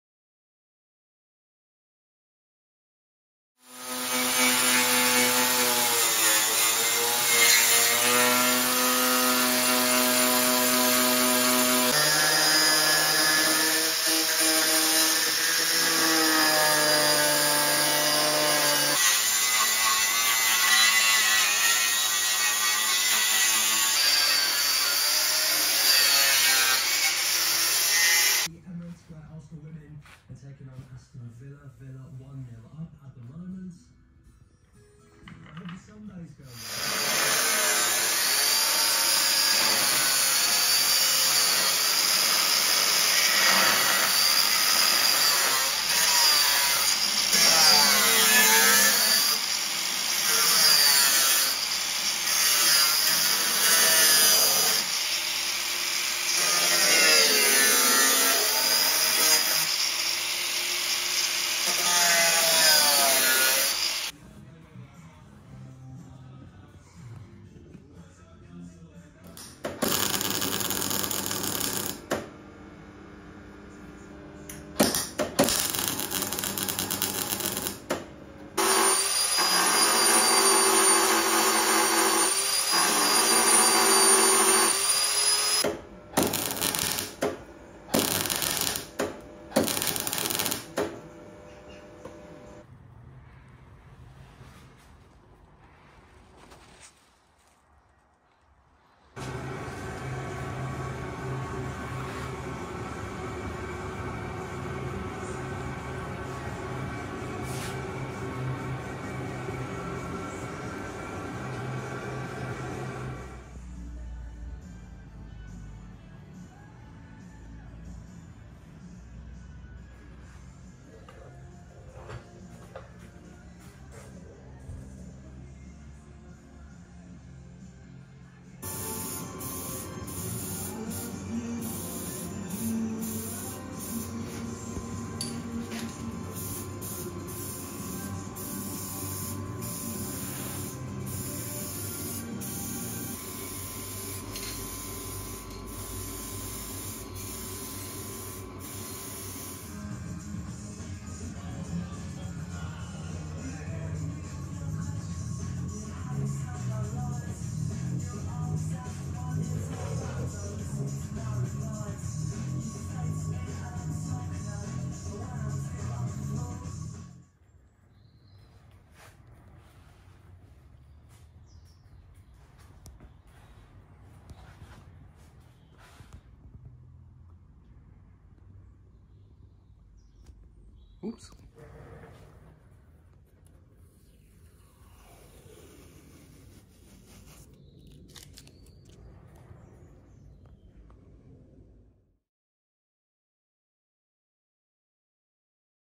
Repairing the rusted door sill on my 2003 Jaguar S-Type R
Video showing the repair of the rusted door sill on the passenger side of my 2003 Jaguar S-Type R.